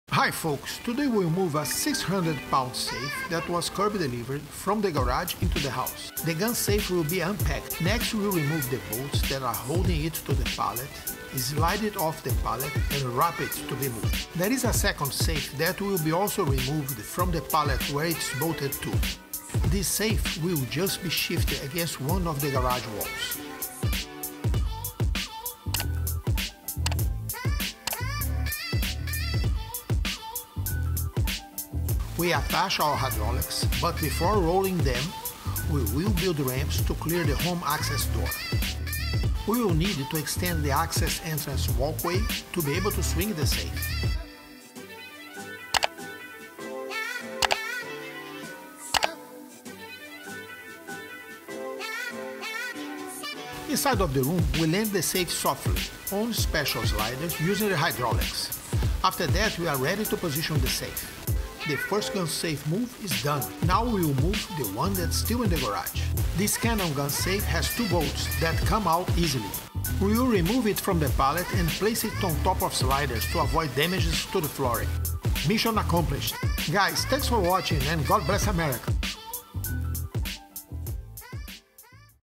MOVING AND INSTALLING A 600 LB REVERE GUN SAFE THAT WAS CURB DELIVERED INTO THE HOUSE
Hi Folks! Today we will move a 600lb safe, that was curb delivered, from the garage into the house.
The gun safe will be unpacked.
Next, we will remove the bolts that are holding it to the pallet, slide it off the pallet and wrap it to be moved.
There is a second safe, that will be also removed from the pallet where it is bolted to. This safe will just be shifted against one of the garage walls.
We attach our hydraulics, but before rolling them we will build ramps to clear the home access door.
We will need to extend the access entrance walkway to be able to swing the safe.
Inside of the room we land the safe softly, on special sliders using the hydraulics. After that we are ready to position the safe.
The first gun safe move is done. Now we will move the one that is still in the garage.
This Canon gun safe has 2 bolts that come out easily.
We will remove it from the pallet and place it on top of sliders to avoid damages to the flooring.
Mission accomplished!

Created and Published by Triumph Services Corporation – Help2move.com

TIMESTAMPS:
00:00 - Introduction
00:07 - Unboxing the 2 safes
00:20 - Wrapping the safe
00:32 - Attaching the jacks
00:36 - Placing ramps to be able to bring the safe into the house
00:50 - Moving the safe inside of the house
00:54 - Positioning the safe and removing the jacks
01:00 - Positioning the first safe in its place
01:06 - Placing the second safe in its place
01:16 - Job done!

VLOG 229